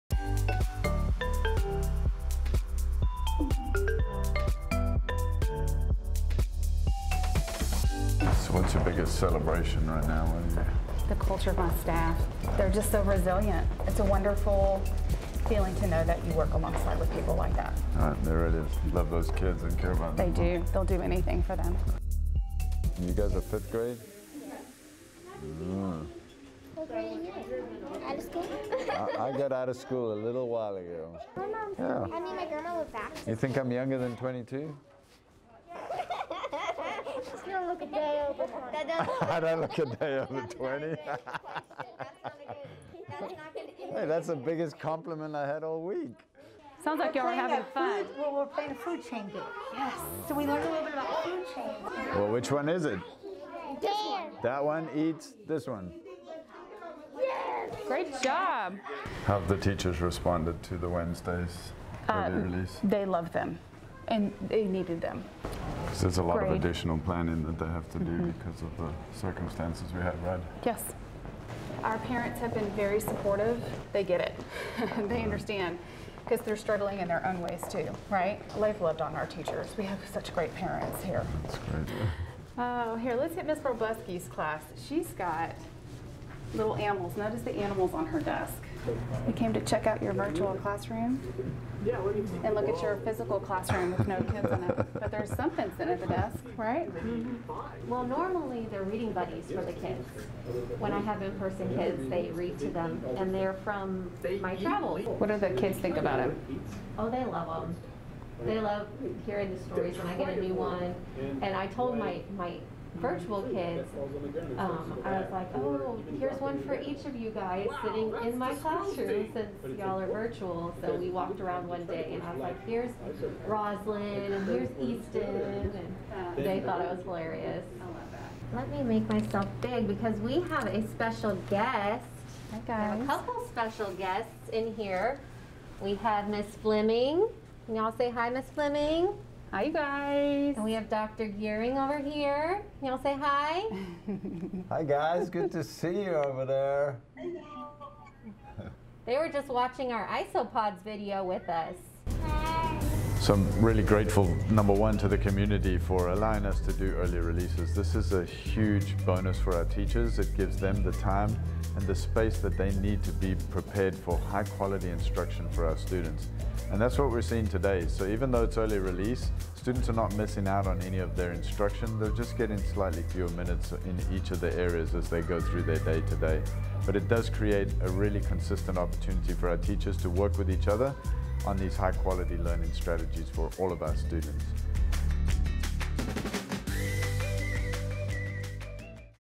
Super School Visit: Camacho Elementary
Days might be cut short on Wednesdays, but that doesn't mean learning is! At Camacho Elementary, the strength of great teachers is packing great material into less time. Enjoy a tour through in-person and virtual classrooms with Superintendent Bruce Gearing, Ed.D. and Camacho principal Gena Fleming.